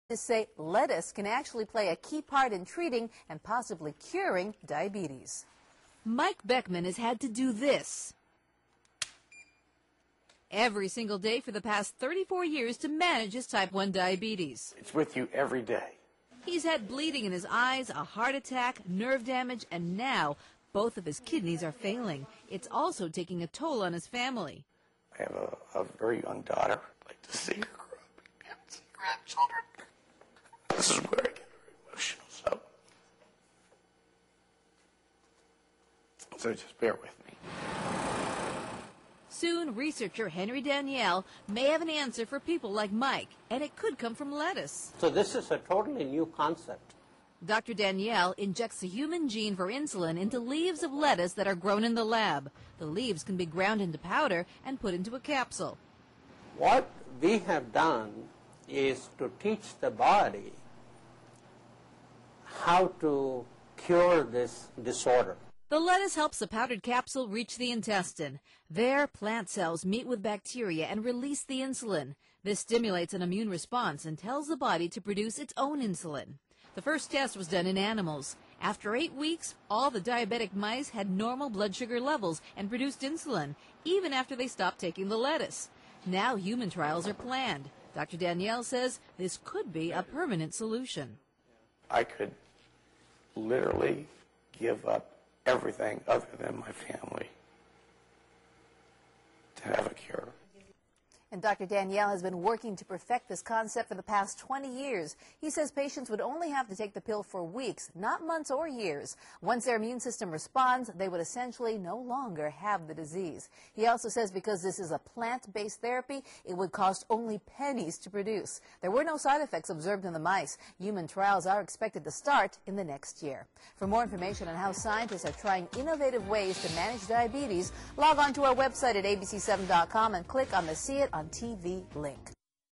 UCF Researchers Create Lettuce That Makes Insulin
In Dr. Daniell's method, the lettuce plant cells help the insulin reach the intestine. Once the plant cells get there, bacteria slowly break down the cell walls and gradually release insulin into the bloodstream. This creates an immune response in the body and teaches it to release its own insulin.
Dr. Daniell says because this is a plant-based therapy, it would only cost pennies to produce.